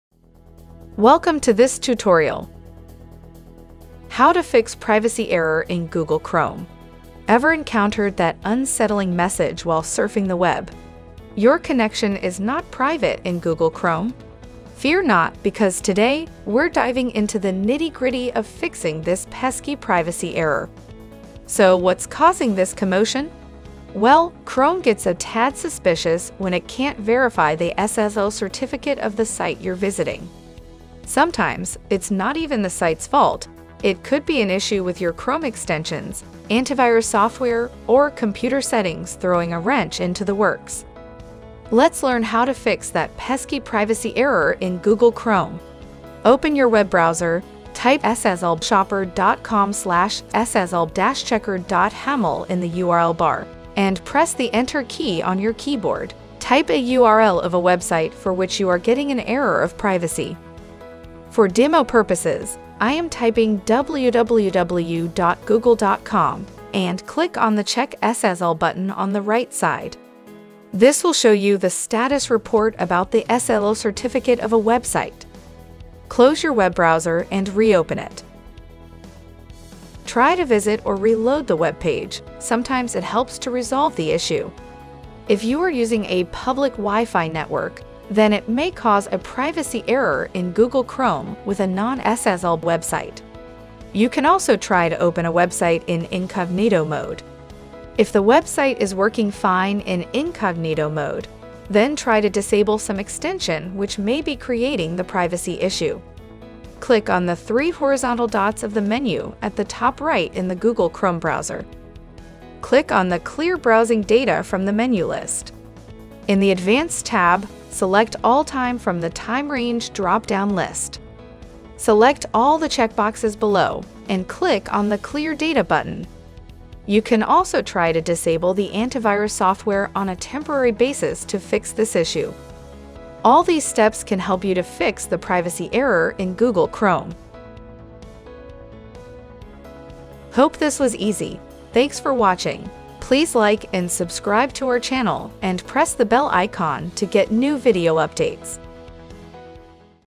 How to Fix Privacy Error in Google Chrome :Tutorial
How to Fix “Your Connection Is Not Private” Error
Ever encountered that unsettling message while surfing the web – "Your connection is not private" in Google Chrome? Fear not, because today, we're diving into the nitty-gritty of fixing this pesky privacy error!
So, what's causing this commotion? Well, Chrome gets a tad suspicious when it can't verify the SSL certificate of the site you're visiting. Sometimes, it's not even the site's fault; it could be an issue with your Chrome extensions, antivirus software, or computer settings throwing a wrench into the works.
Let's learn how to fix that pesky privacy error in Google Chrome!
1. Open your web browser,
2. Type in the URL bar, and press the Enter key on your keyboard.
3. Type a URL of a website for which you are getting an error of privacy.
4. For demo purposes, I am typing and click on the Check SSL button on the right side.
5. This will show you the status report about the SSL certificate of a website.
6. Close your web browser, and reopen it.
7. Try to visit or reload the web page, sometimes it helps to resolve the issue.
8. If you are using a public Wi-Fi network, then it may cause a privacy error in Google Chrome with a non-SSL website.
9. You can also try to open a website in Incognito mode.
10. If the website is working fine in Incognito mode, then try to disable some extension which may be creating the privacy issue.
11. Click on the three horizontal dots of the menu at the top right in the Google Chrome browser.
12. Click on the Clear browsing data from the menu list.
13. In the Advanced tab, select All time from the Time Range drop-down list.
14. Select all the checkboxes below, and click on the Clear data button.
15. You can also try to disable the Antivirus software on a temporary basis to fix this issue.
16. All these steps can help you to fix the privacy error in Google Chrome.